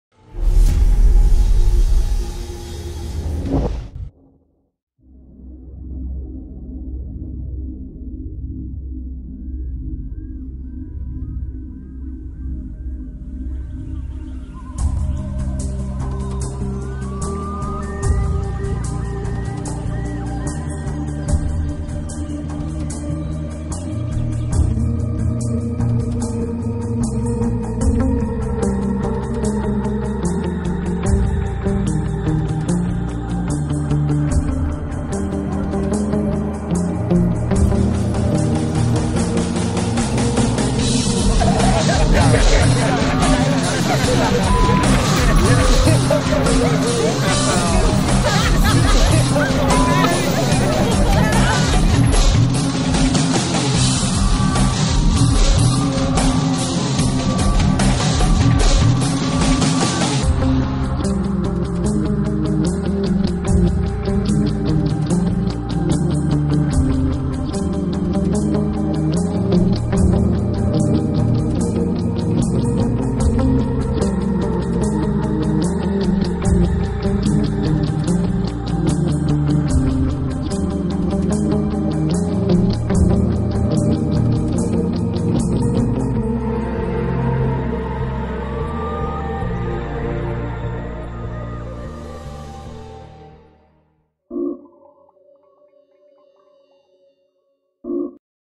Smoke Magic
Cool Magic that you can produce smoke from your mouth anytime, anywhere!